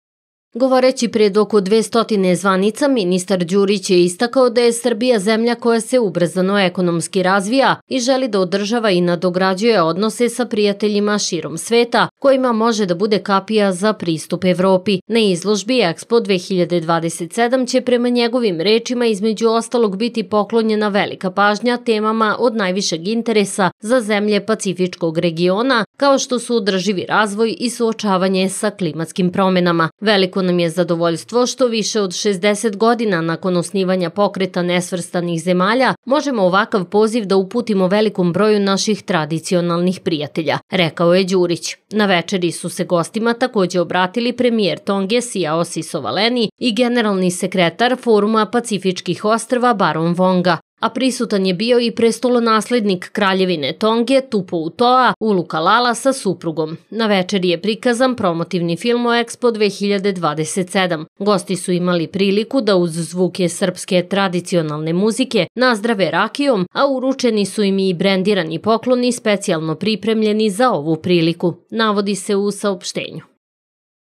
Đurić u Tongi upriličio večeru za učesnike Foruma pacifičkih ostrva: Pred 200 zvanica o Ekspo 2027
Ministar spoljnih poslova Srbije Marko Đurić priredio je, na marginama sastanka lidera Foruma pacifičkih ostrva u Tongi, večeru za učesnike i goste tog skupa, na kojoj je predstavio pripreme Beograda za izložbu Ekspo 2027, saopštilo je ministarstvo spoljnih poslova.